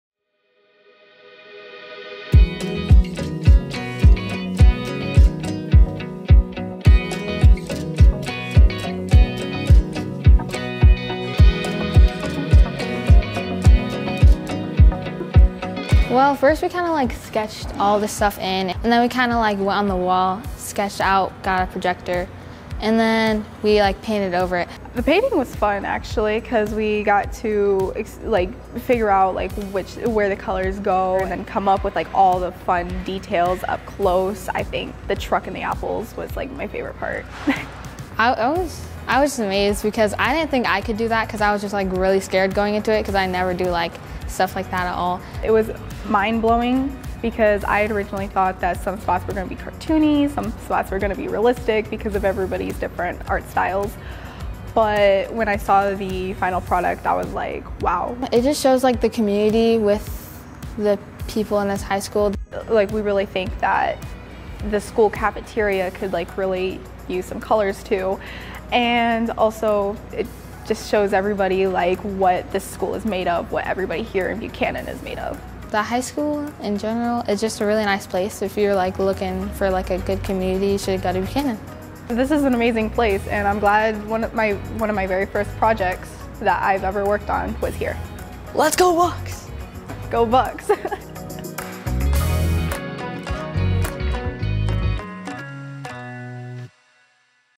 BHS Cafeteria Mural
The high school kitchen manager had the idea to have students paint a mural on what was a large white wall with a maroon zigzag stripe across it. Mrs Miller, the high school art teacher offered designing it as an optional project for art class and a number of kids submitted their ideas. There was a committee look them over and final design was a combination of aspects from a few different submissions.